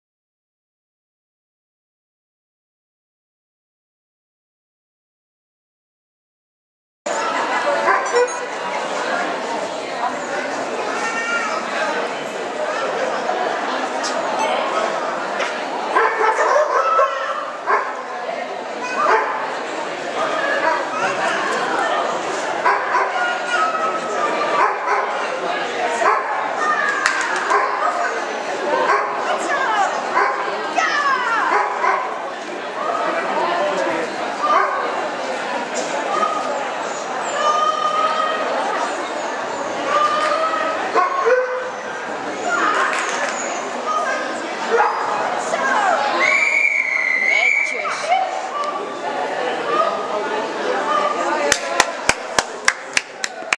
Tolbert 22 02 09 Behendigheidswedstrijd, Gambling Leida en Sam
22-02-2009
Behendigheidswedstrijd te Tolbert. Sam is een beginnend hondje (kleine kees) in de wedstrijdwereld van behendigheid.  Alles is leuk en nieuw voor hem.
Helaas moeten we starten in de A-mini klasse en niet in de debutantenklasse omdat ik al met Bootsman (grote witte keeshond) in de B-midi loop.
Ondanks zijn onervarenheid behaalden wij de vijfde plaats op de gambling A-mini. Best een goed resultaat.